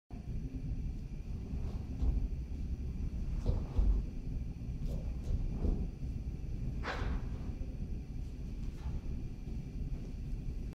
Basics: Backfist in cross stance
Master Meggie demonstrates backfist in cross stance at Core Taekwondo's San Mateo facility.